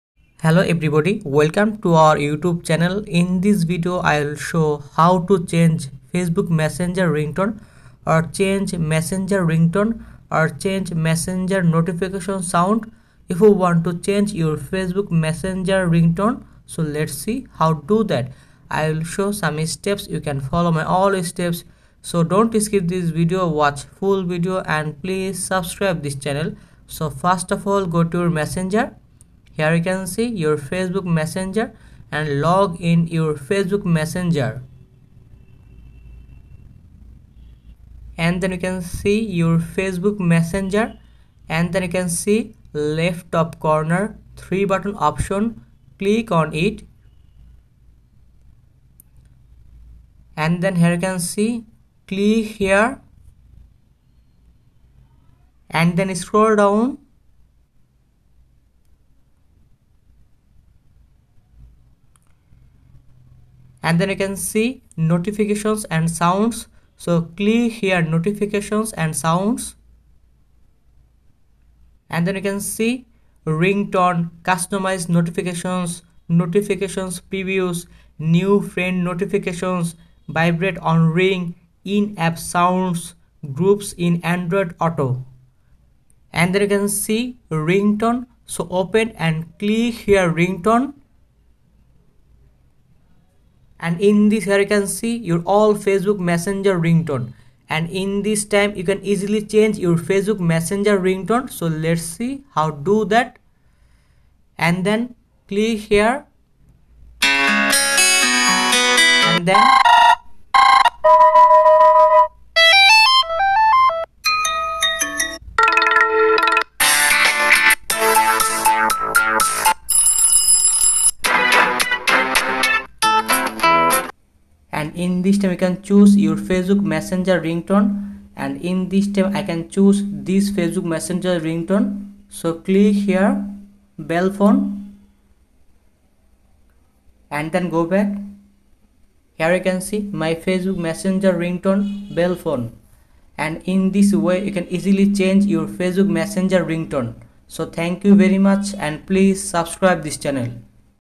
How To Change Ringtone On Facebook Messenger
In This Video I Will Show How To Change Ringtone On Facebook Messenger.
If You Want Change Your Messenger Ringtone You Can Follow This Video All Steps & Then You Can Easily Change Your Facebook Messenger Ringtone.